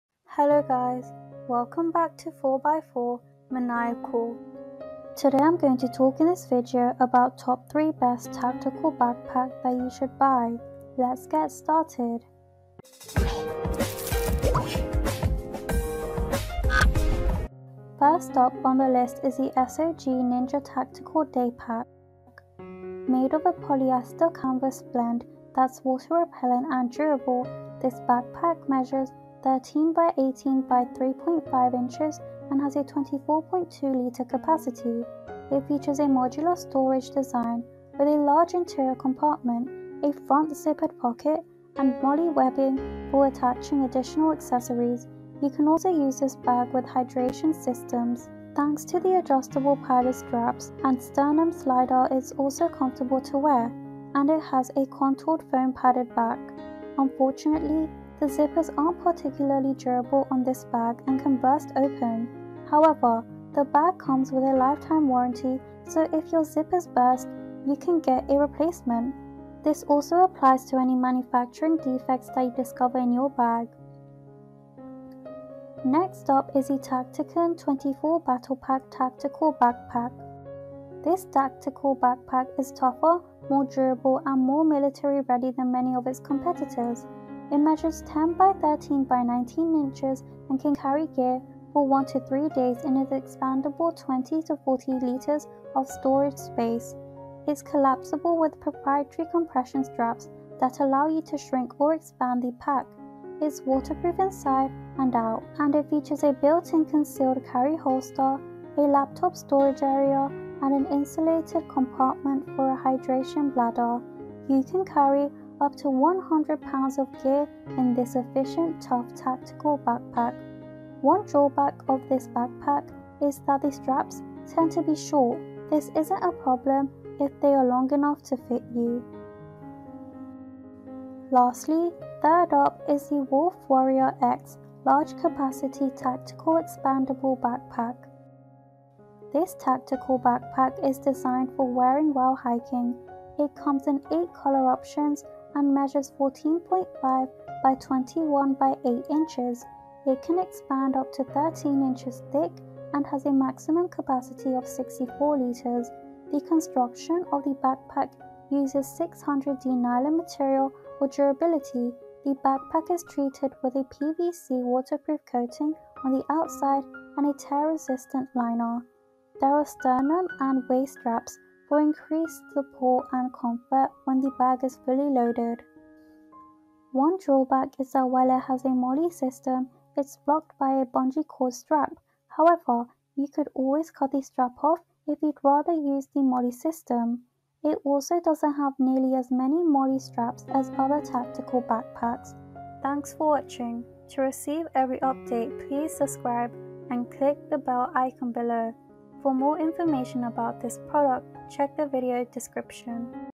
✅ 3 Best EDC Tactical Backpack - Heavy Duty Tactical Backpack
A tactical backpack is one of the most basic pieces of tactical gear because it allows you to easily carry your gear, clothing, and essentials.

SUPPORT 4x4Maniacal

# tacticalbackpack